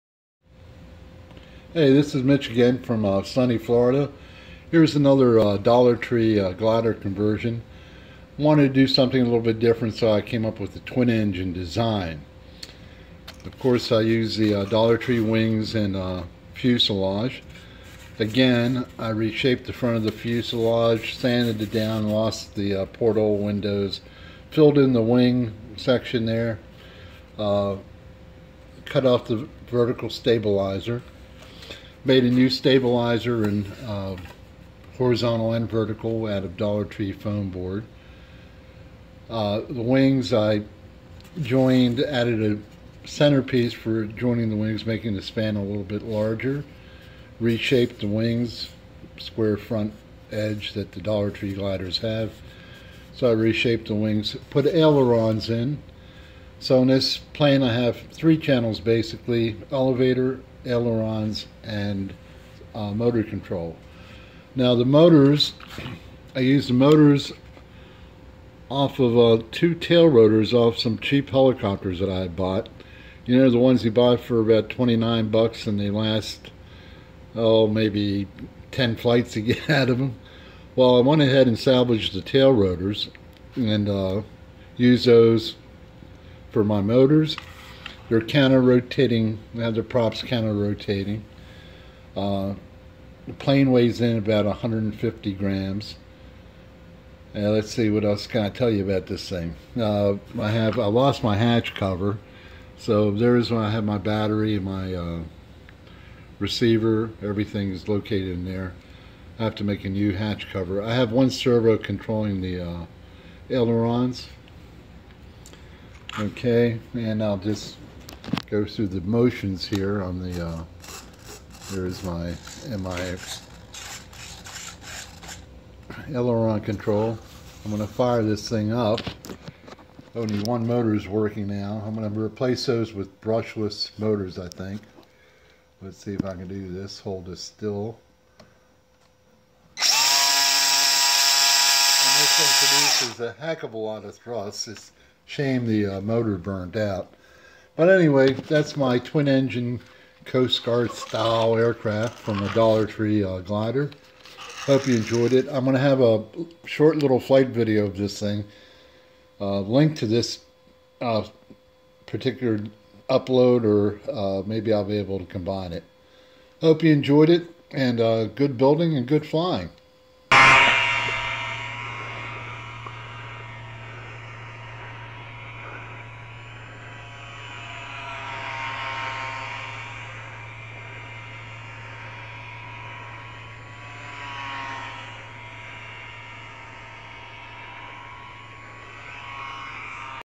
Dollar Tree Glider Conversion to Twin Electric RC
Wingspan: 26 inches
Flying Weight: 150 Grams
Twin Geared Brushed Electric
3 Channel, 2s 300 mah lipo